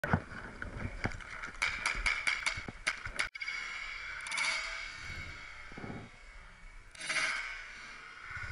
Lost Barrel Insert
Don't ever take apart your Superfreak Barrel kit in the middle of a game. I didn't even realize it happened until the round ended.